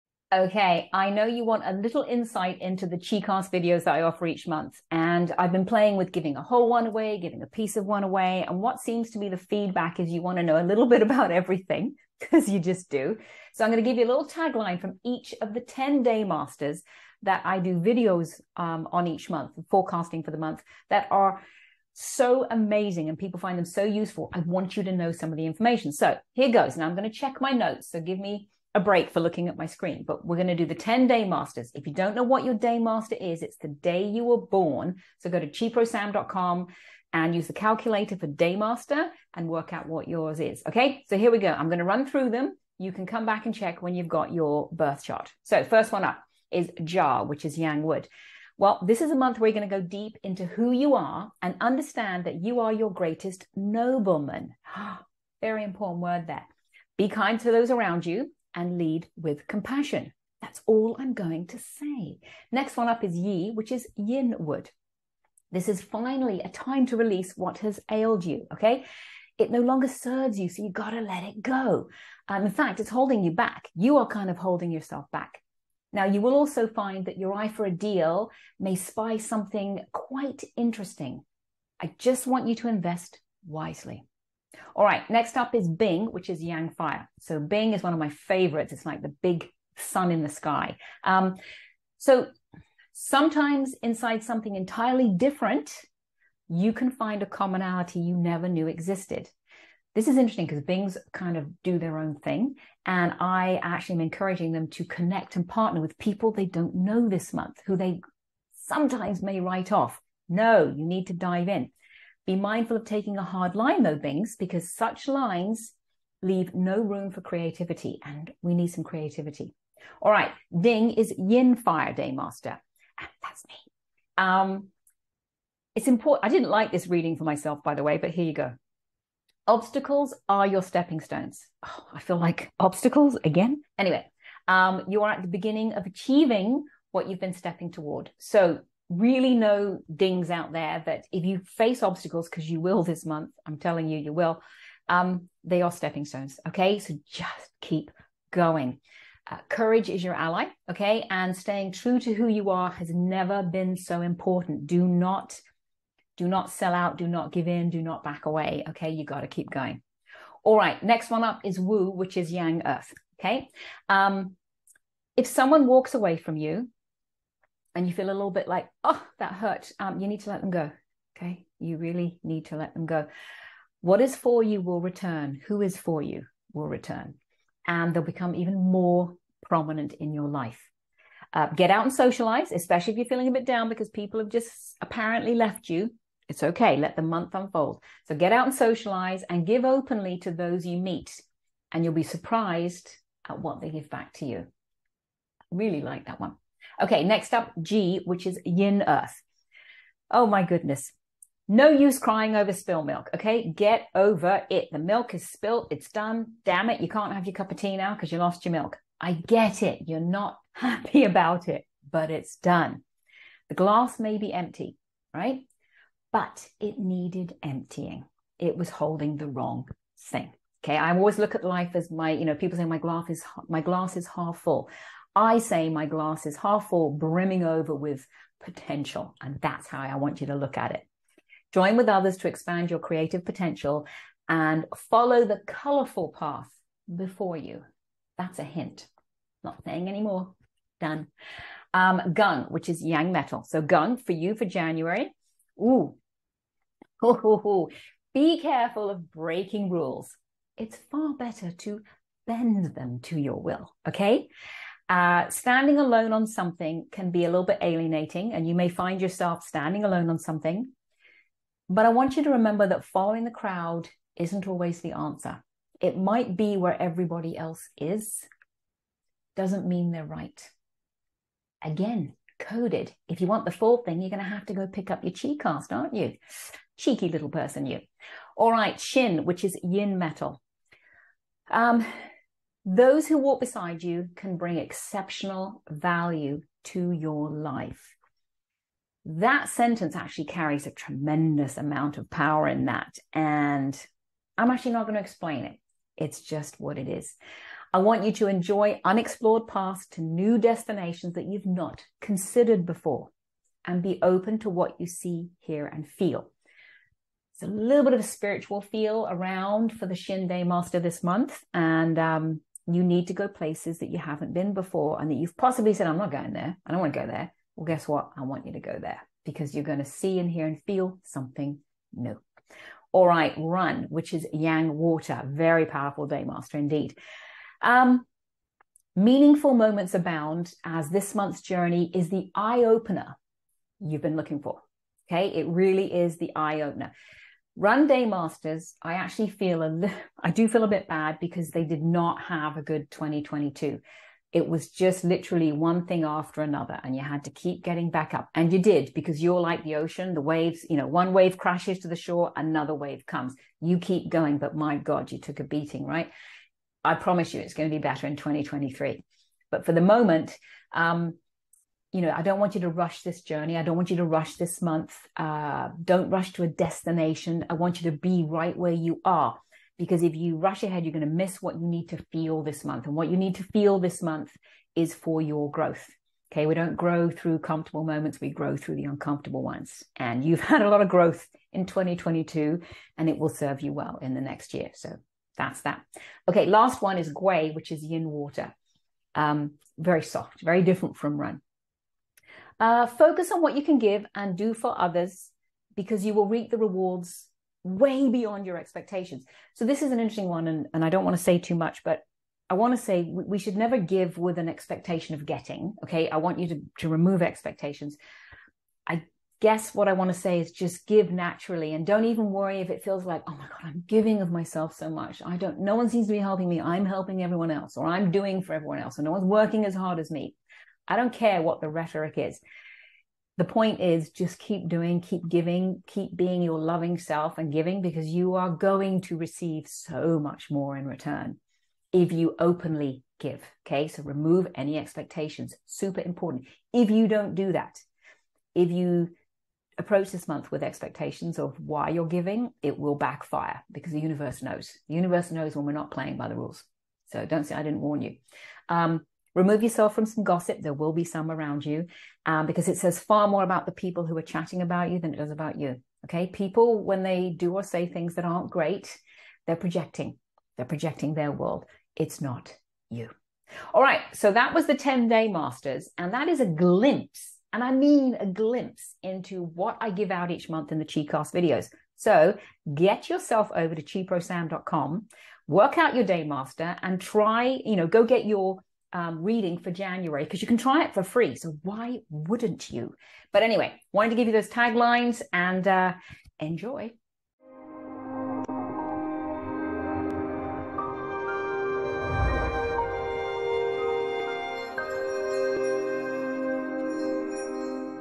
January 2023 Day Master Forecast
Find out what January has in store for you based on the day of your birth ~ the ONLY and BEST way to know your forecast!

For the FULL forecast, pop over here and try for FREE